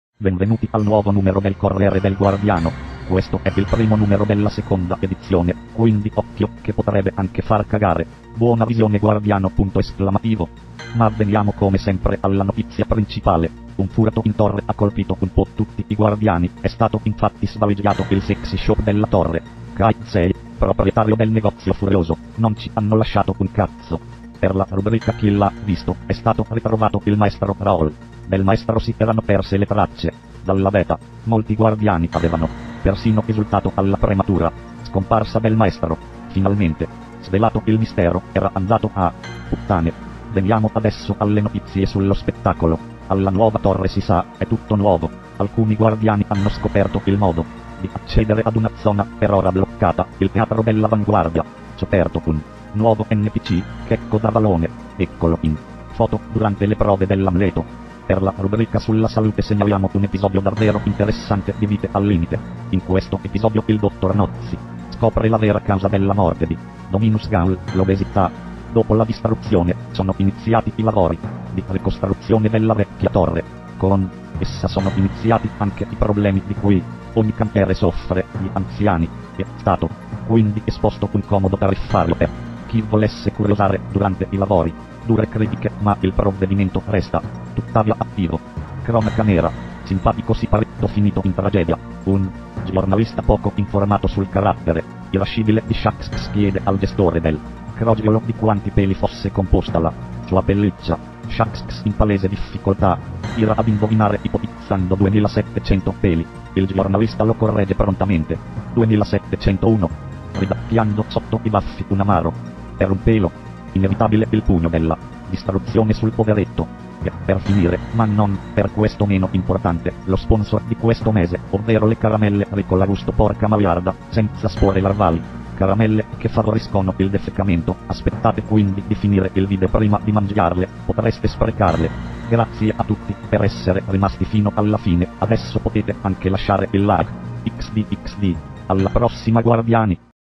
Corriere del guardiano 2 - Anno 1 Numero 1
Finalmente la nuova versione del corriere del guardiano, arrivato alla seconda versione. Ho duvuto usare loquendo perchè non ho al memento possibilità di usare il microfono per registrare.

Grazie per il supporto in anticipo guardiani!